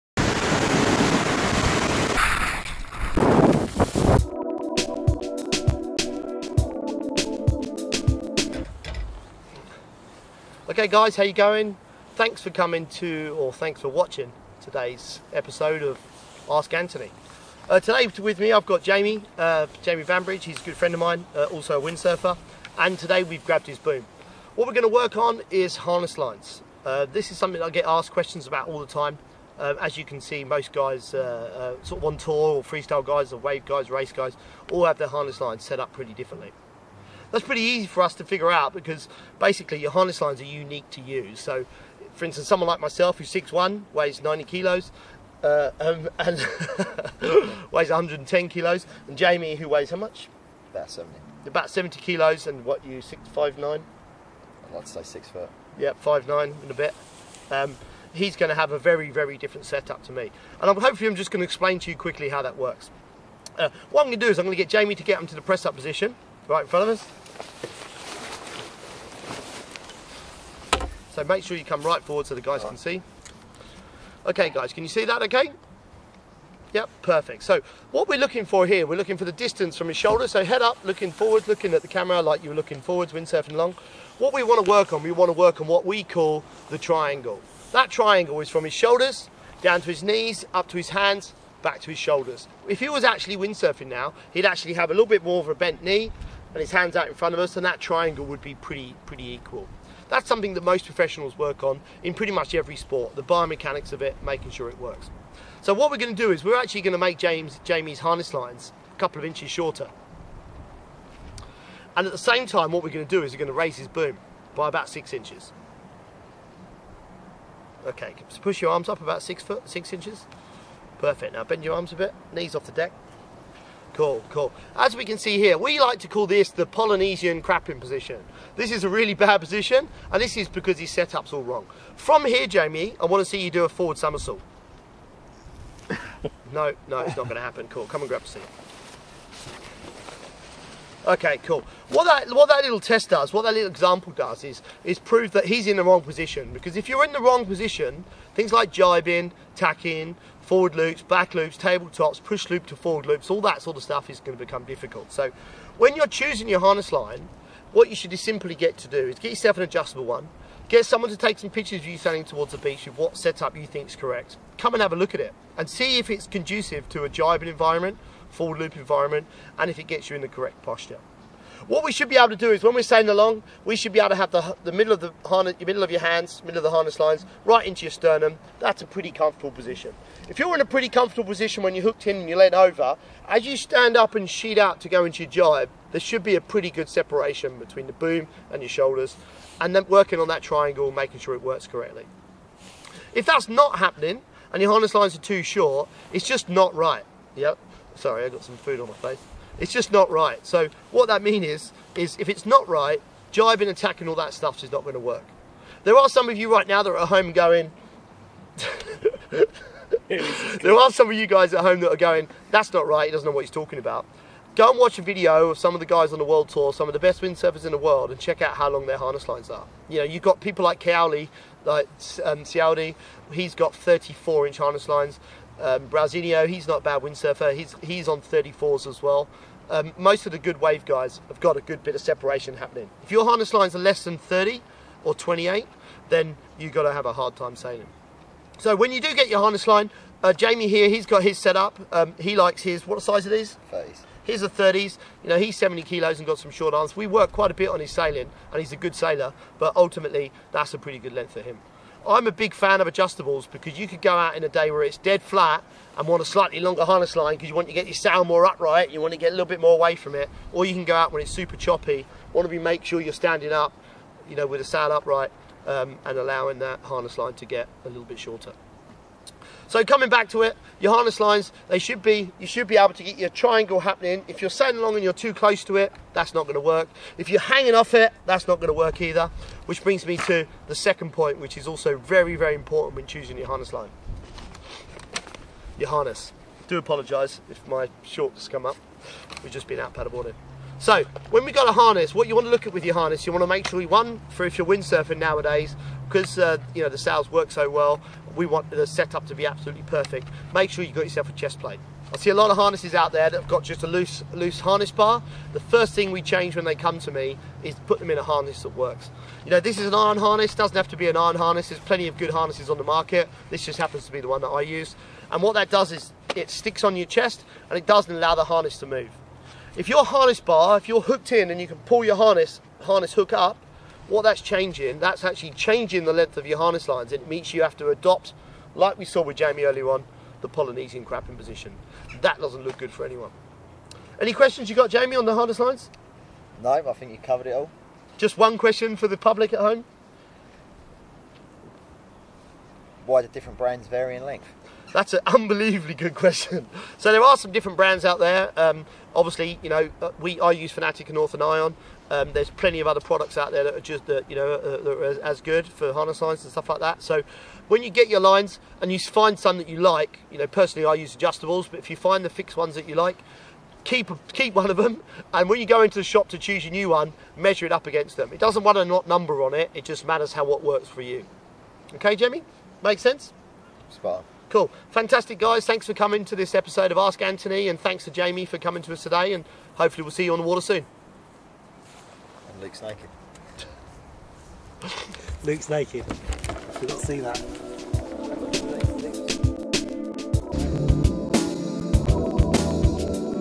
Ask Ant Series - Harness Lines
is this new episode of 'Ask Ant' we cover harness lines and what you shoul be doing :)